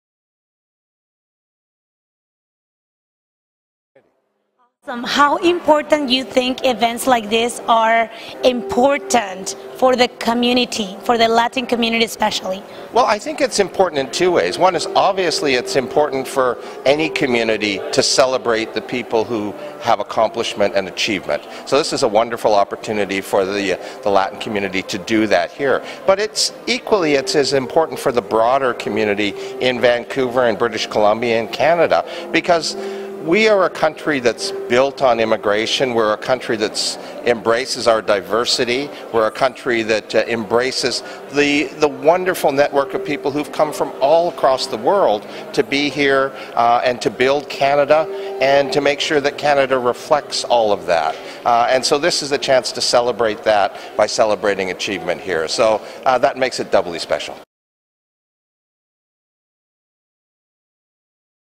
Inspirational Latin Awards 2018
At Latincouver, empowering people has always been one of our priorities.
The Inspirational Latin Awards is our way of recognizing the outstanding achievements many individuals living in BC have accomplished. We commemorate individuals from who have made a significant difference in their community. They have guided, supported and encouraged their business. They have been considered an asset in the development and commitment of the community they live in.
This year, we have something extraordinary happening.
We are celebrating together with the “10 most influential Hispanic Canadians Awards”.